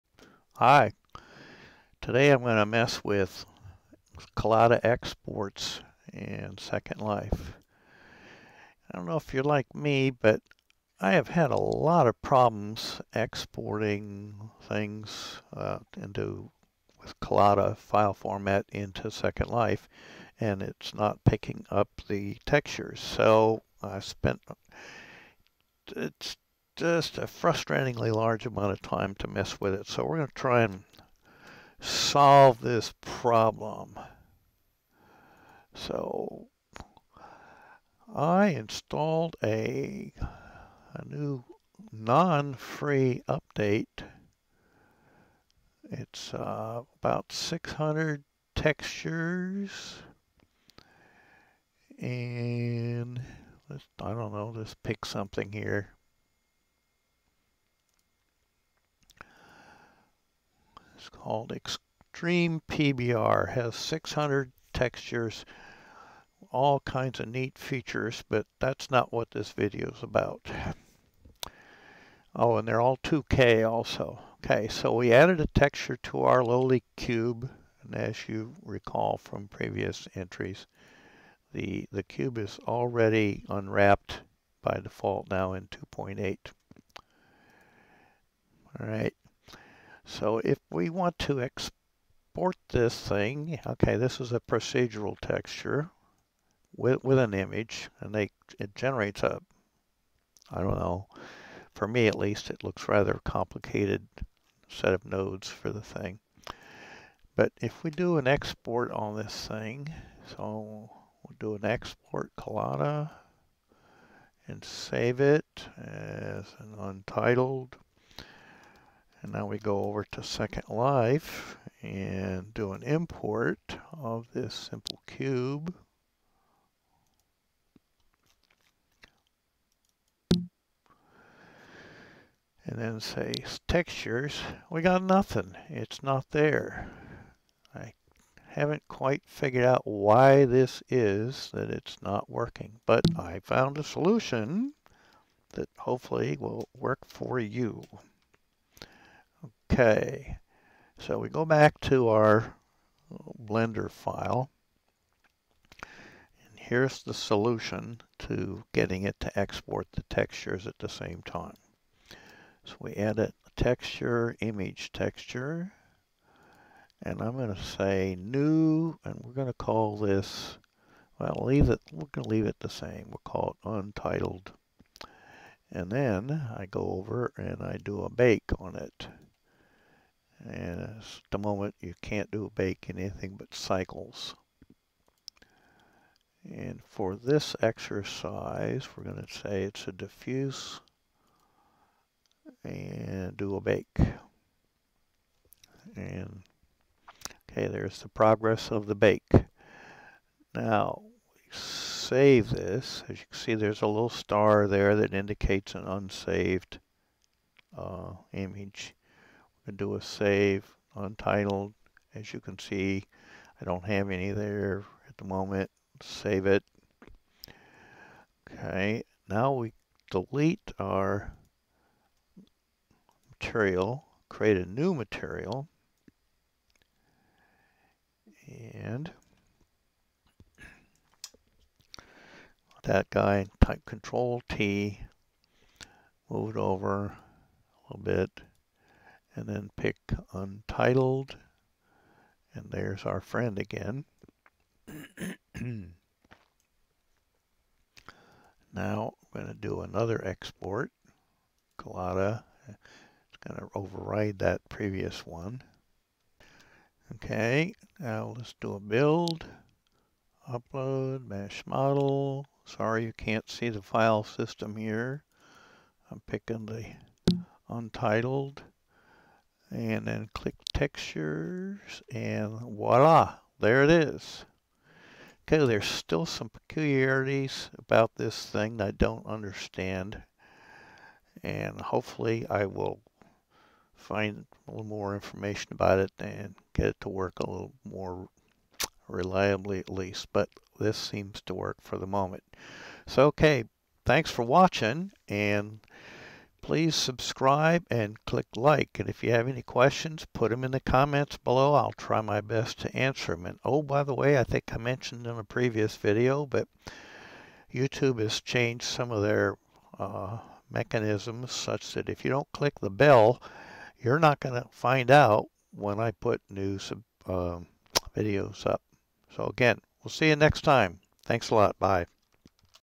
#026 Export Collada with textures for SL
Get Blender 2.8 to include your textures in the collada file with this procedure.